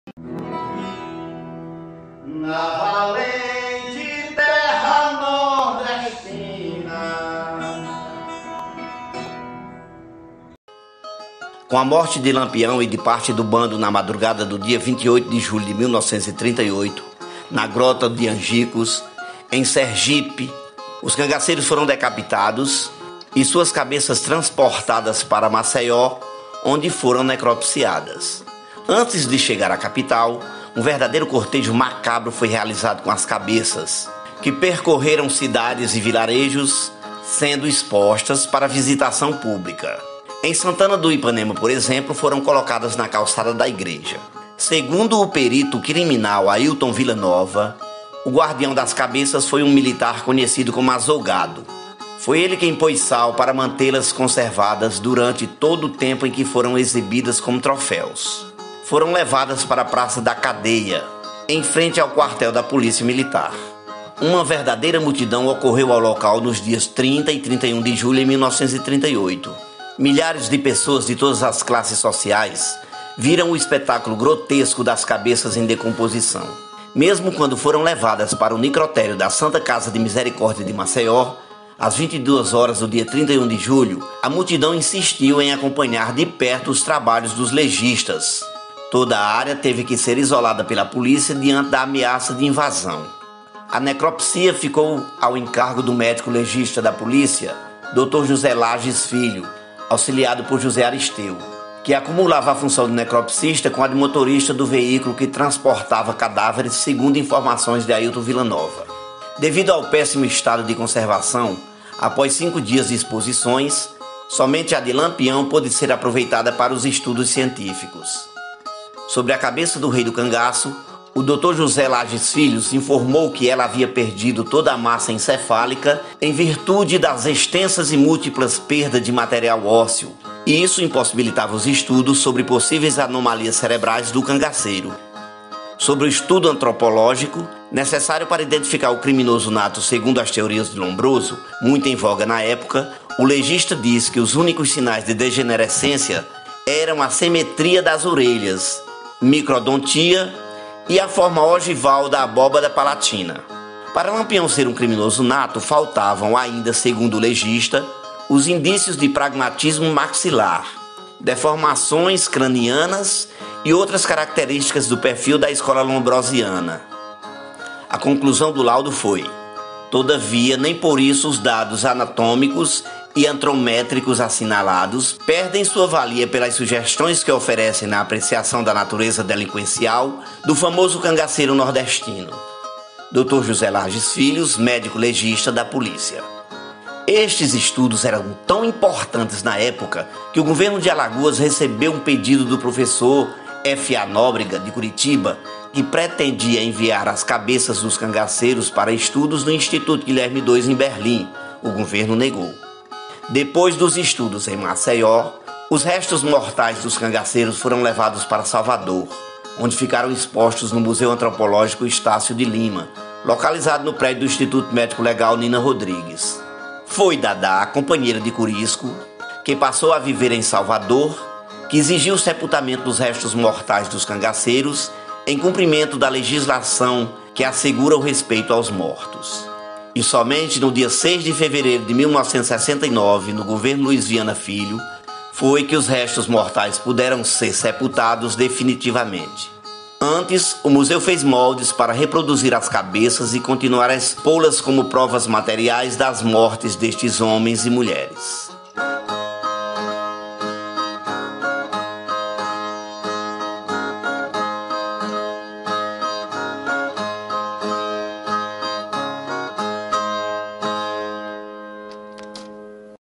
A CABEÇA DE LAMPIÃO
Com a morte de Lampião e de parte do seu bando na madrugada do dia 28 de julho de 1938, na grota de Angicos, em Sergipe, os cangaceiros foram decapitados e suas cabeças transportadas para Maceió onde foram necropsiadas.

Antes de chegar à capital, um verdadeiro cortejo macabro foi realizado com as cabeças, que percorreram cidades e vilarejos, sendo expostas para visitação pública.